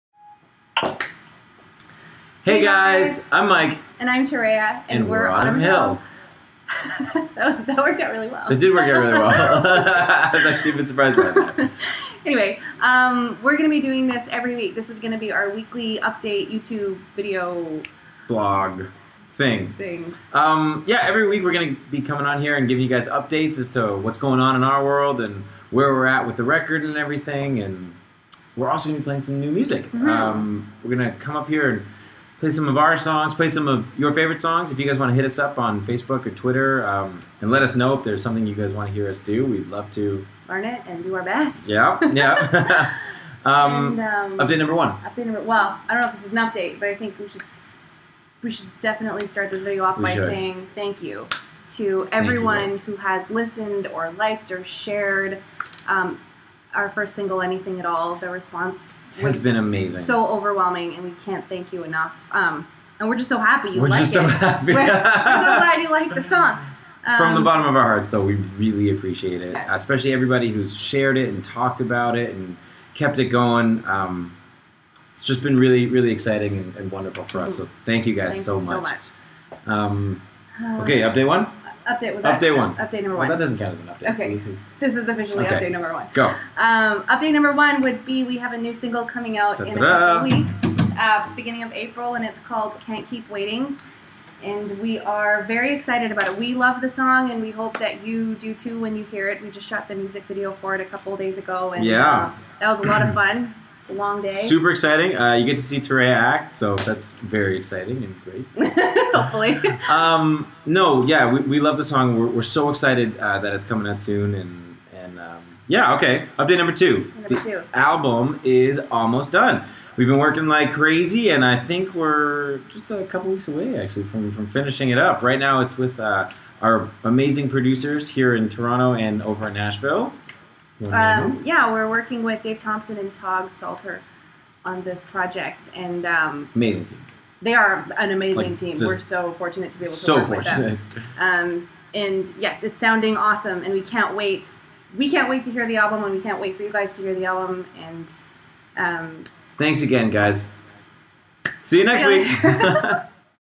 Autumn Hill Video Blog #1
Hi everyone! Here is our very first video blog! We're going to keep these coming every week to keep all of you in the loop on whats new with Autumn Hill. We laugh a lot and occasionally say the same thing at the same time. Enjoy!